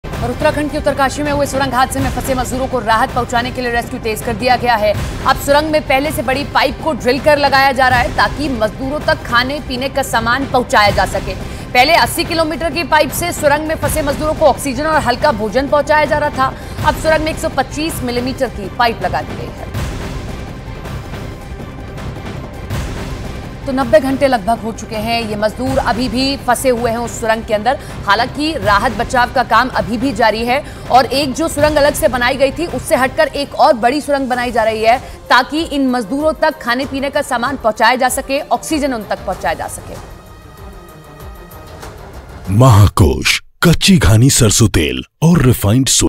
Breaking News : सुरंग में फंसे मजदूरों को राहत पहुंचे के लिए रेस्क्यू |Uttarkashi Tunnel Collapse
Rescue has been intensified to provide relief to the workers trapped in the tunnel accident in Uttarkashi, Uttarakhand. Now a bigger pipe than before is being drilled and installed in the tunnel so that food items can be supplied to the workers. Earlier, oxygen and light food was being supplied to the workers trapped in the tunnel through 80 mm pipe. Now 125 mm pipe is being installed in the tunnel.
उत्तराखंड के उत्तरकाशी में हुए सुरंग हादसे में फंसे मजूदरों को राहत पहुंचाने के लिए रेस्क्यू तेज कर दिया गया है। अब सुरंग में पहले से बड़ी पाइप को ड्रिल कर लगाया जा रहा है ताकि मजूदरों तक खाने पीने का सामान पहुंचाया जा सके। पहले 80 मिलीमीटर की पाइस से सुरंग में फंसे मजूदरों को ऑक्सीजन औऱ हल्का भोजन पहुंचाया जा रहा था। अब सुरंग में 125 मिलीमीटर की पाइप लगाई जा रही ।